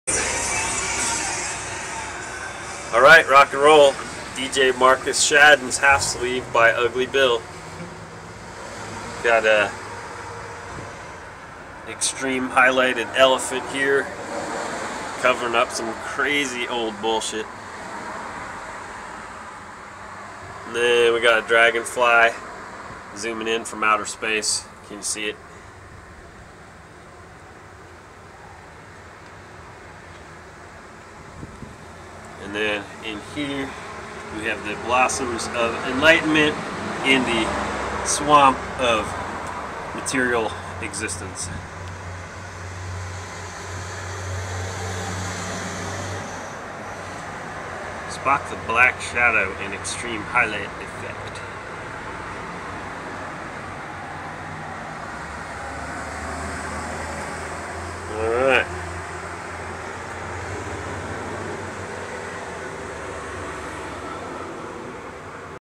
Elephant Half Sleeve by Ugly Bill
This is a tattoo i did several years ago for DJ Marcus Shadden. i never got photos but the video looks cool. I think it has held up pretty well over time...